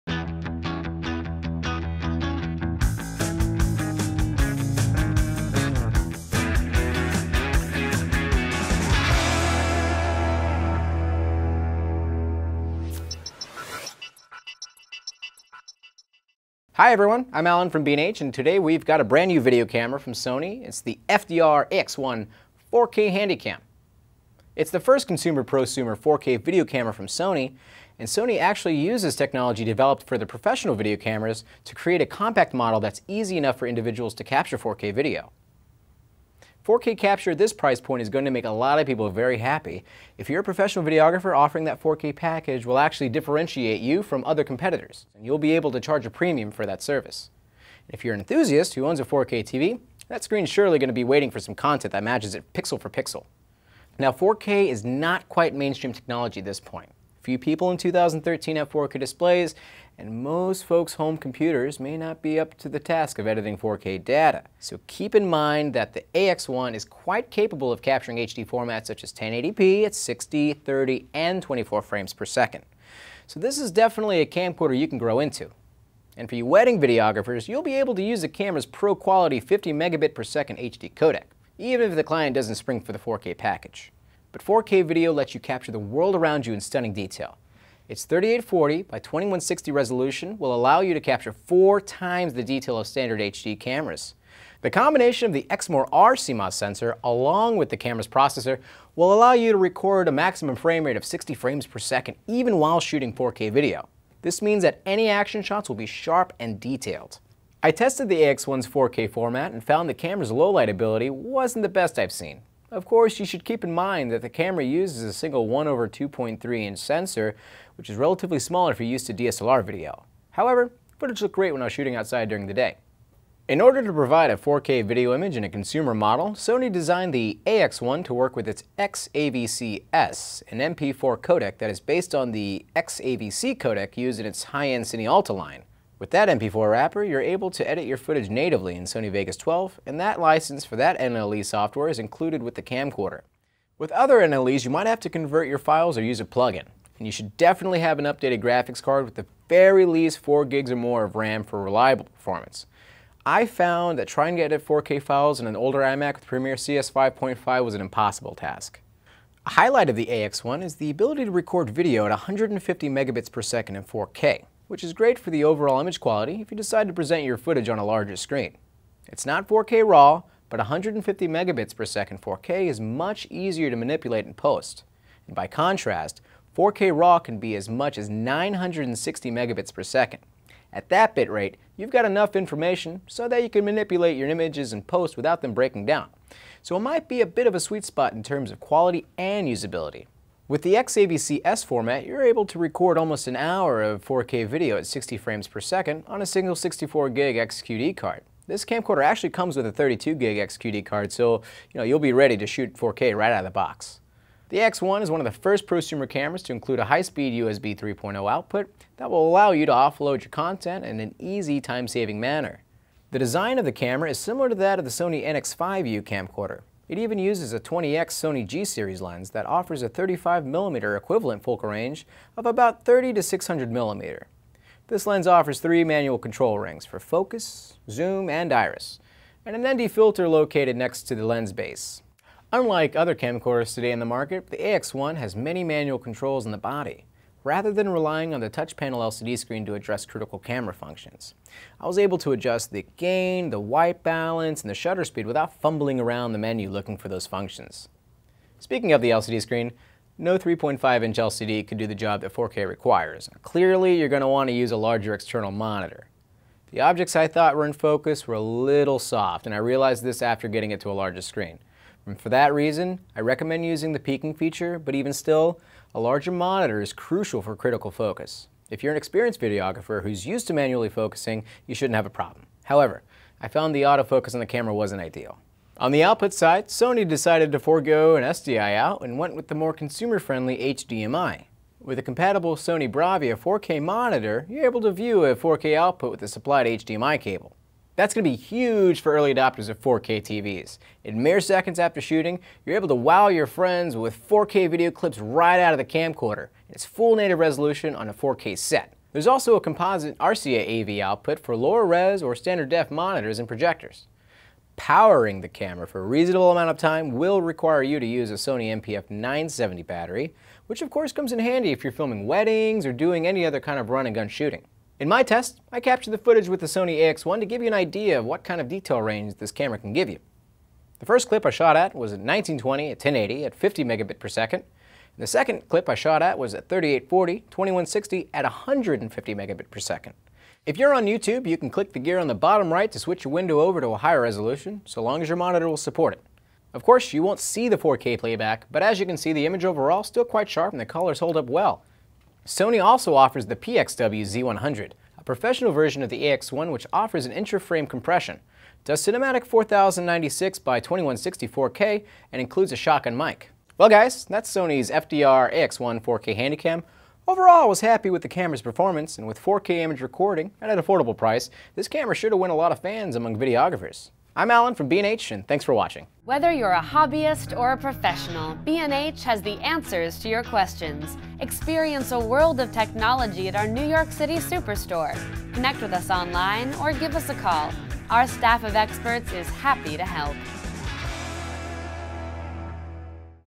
First Look: Sony | FDR-AX1 4K Handycam
The Sony FDR-AX1 Handycam is the first consumer/prosumer camcorder that shoots at stunning 4K resolution at 3840 x 2160. With its 20x zoom lens, the AX1 looks and feels like some of Sony's professional compact HD camcorders, but under the hood, the camcorder packs the XAVC-S codec, which packages high-quality 4K video at manageable bit rates.